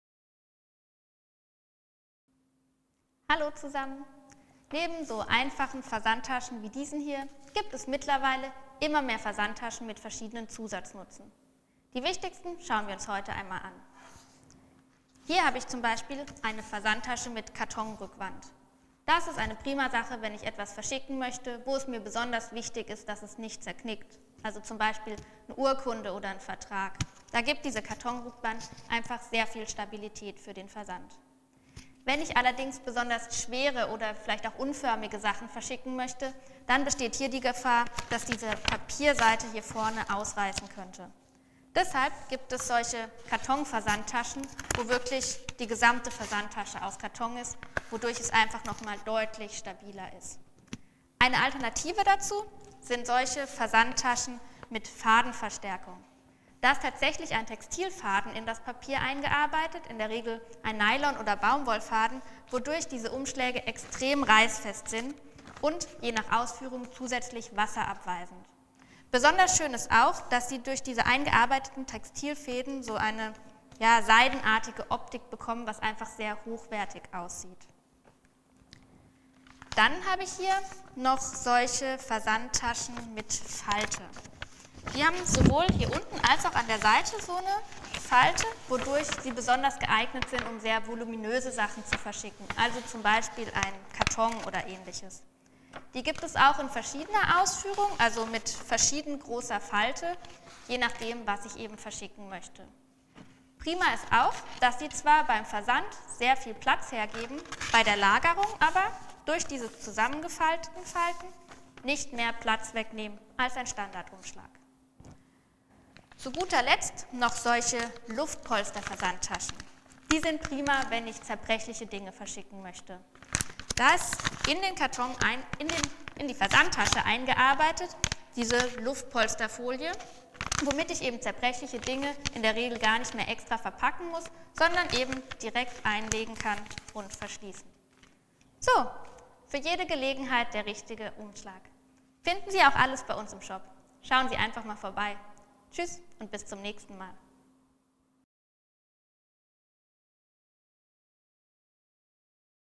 Versandtaschen: Wie kommt meine Post gut an?
Versandtaschen und Umschläge gibt es in verschiedenen Ausführungen. Je genauer die Versandtasche zum Versandgut passt, desto besser kann sie es schützen. Wie Sie ganz schnell die perfekte Versandtasche für Ihre Anforderungen finden, sehen Sie in unserem Video.